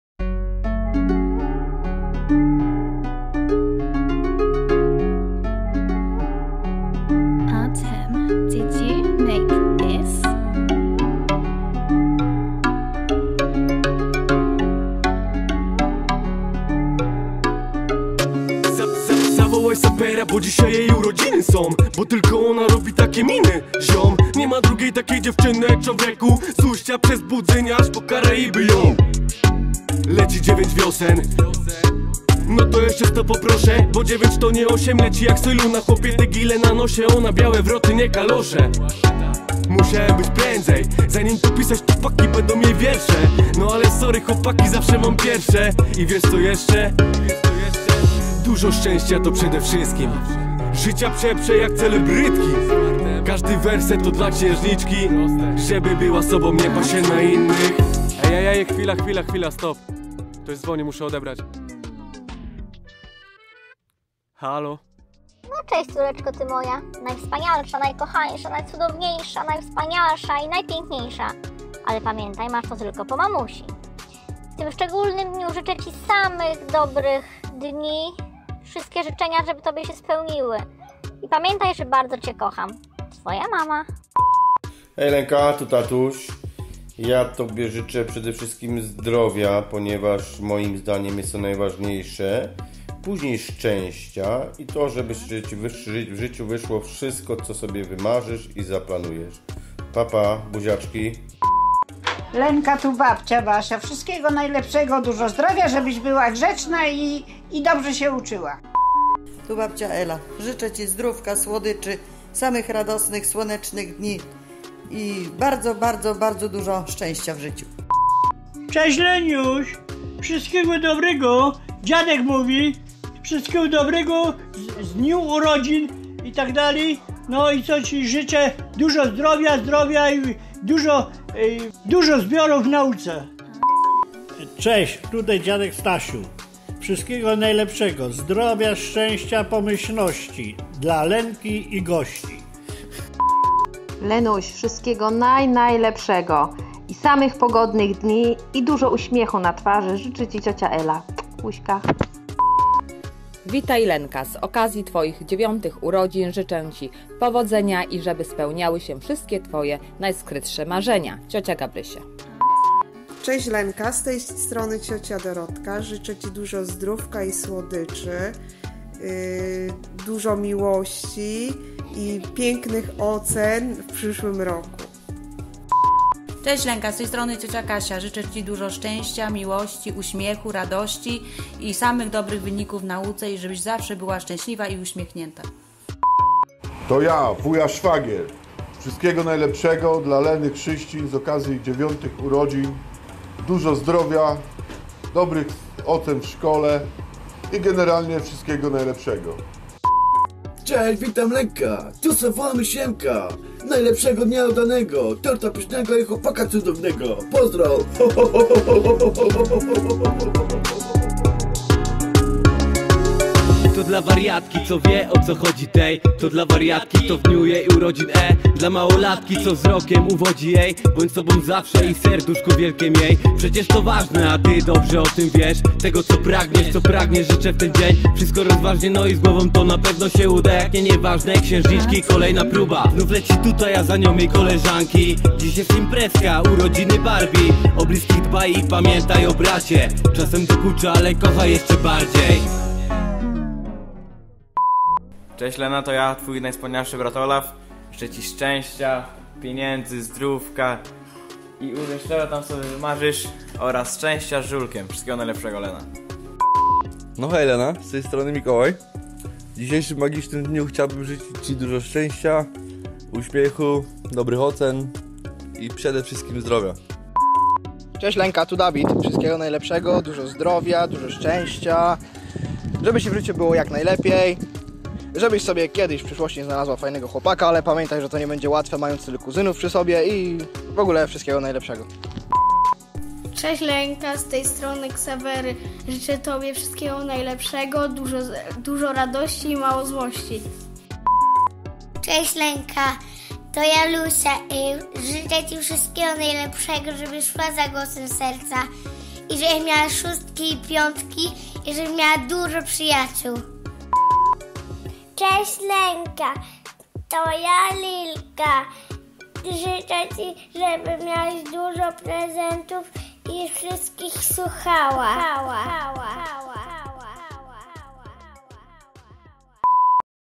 Urodziny Leny
Wszystkiego Najlepszego!
Projekt na 9 urodziny Leny, ktory powstał w skutek współpracy calej rodziny.
Pamietaj Lena wszyscy Cie Kochamy Peace Yo! ;)